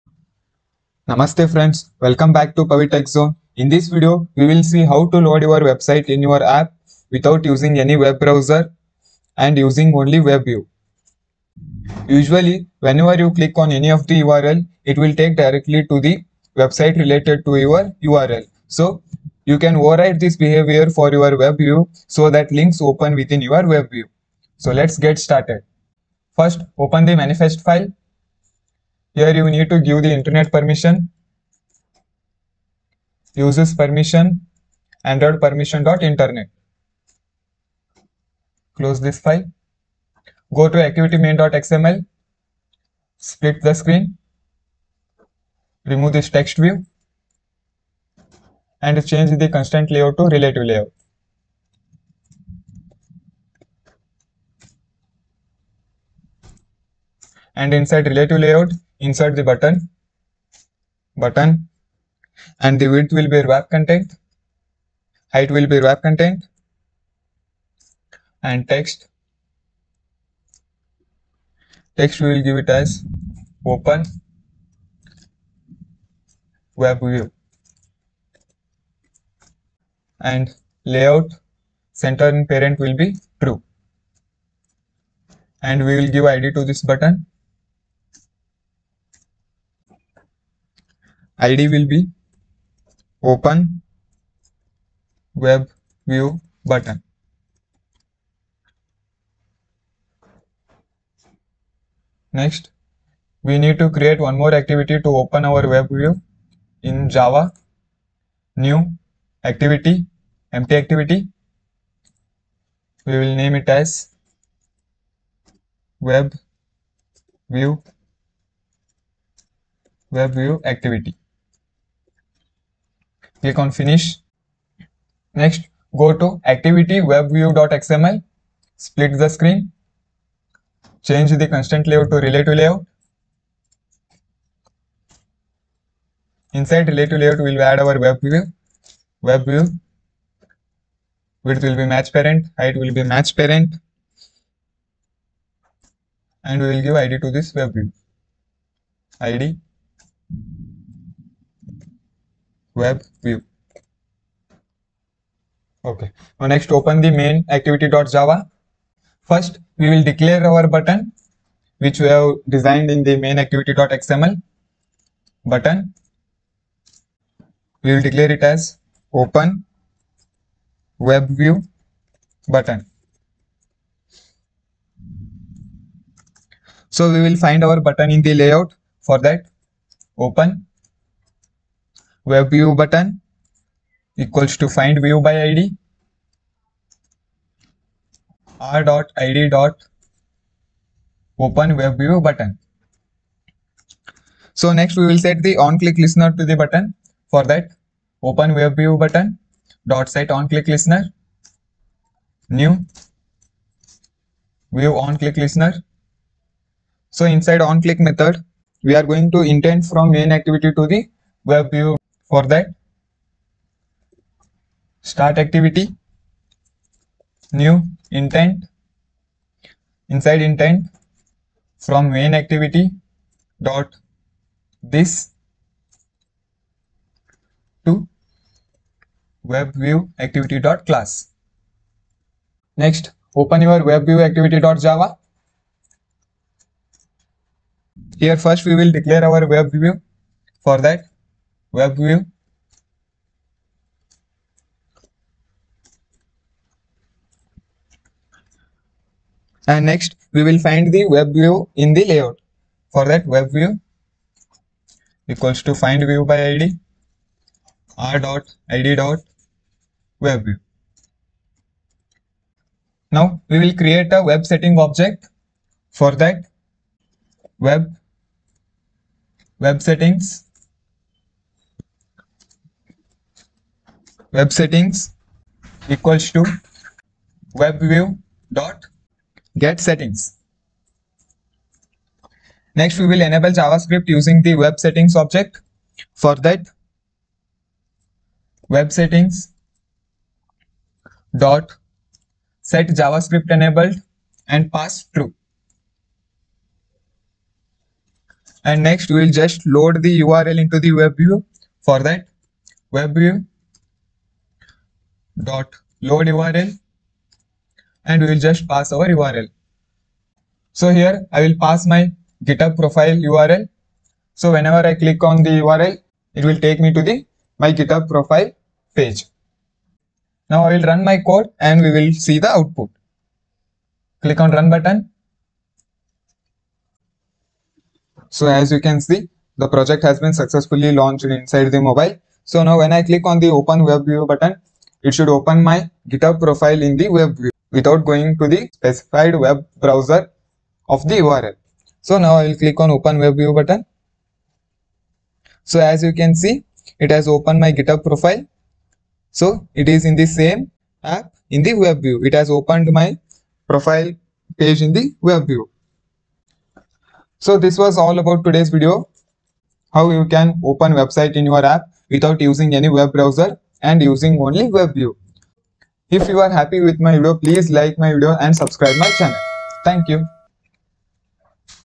Mastering WebViews: Open Your Website in Android App Without a Browser!
Welcome to @Pavi_TechZone Dive into the world of Android Studio as we unravel the mysteries of WebViews. Learn how to seamlessly integrate your website into your Android app without relying on external browsers. From implementation techniques to customization tips, we've got you covered.

Join us for expert insights and step-by-step tutorials, and empower your app development journey with the power of WebViews.